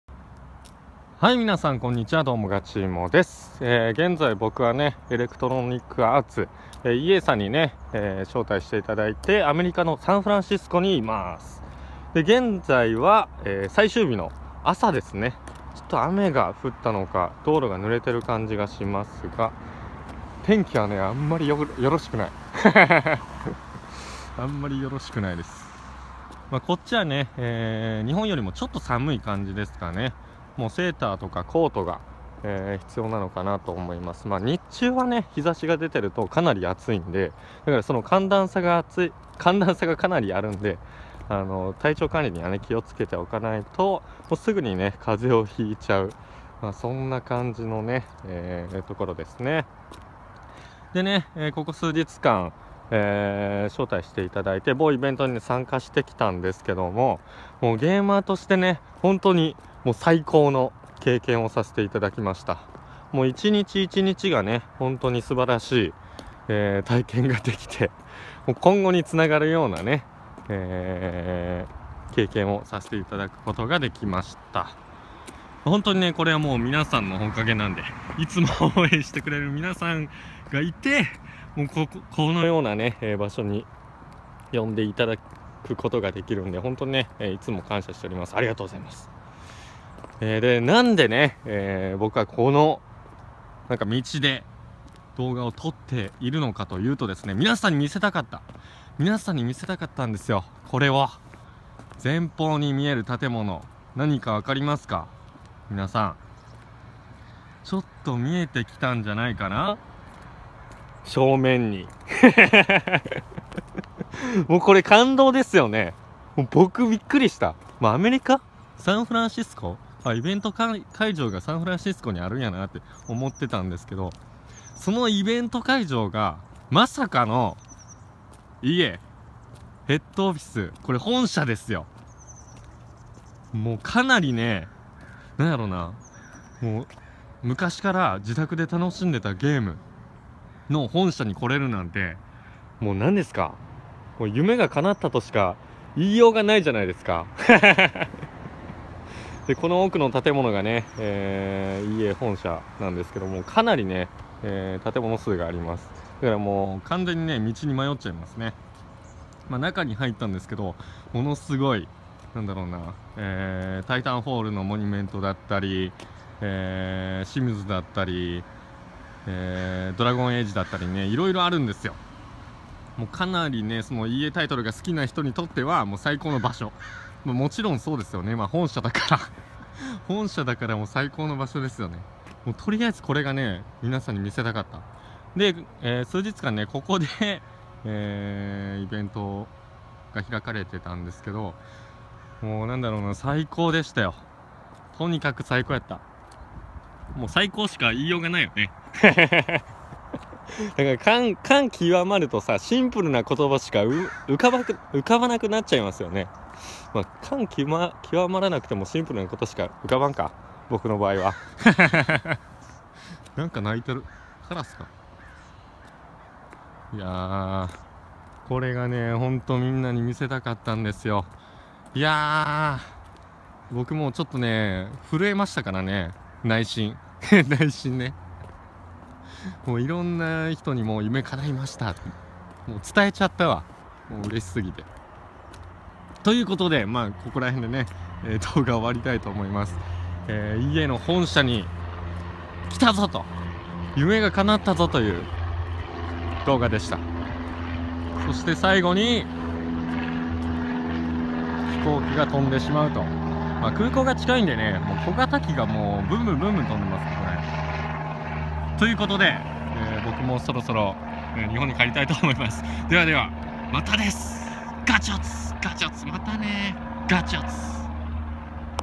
EA本社 夢が叶いました！がち芋 in レッドウッドシティ
※動画内でサンフランシスコと発言していますが、正しくはレッドウッドシティです。
長年の夢が叶いました！！
今まで応援して下さり本当にありがとうございました！！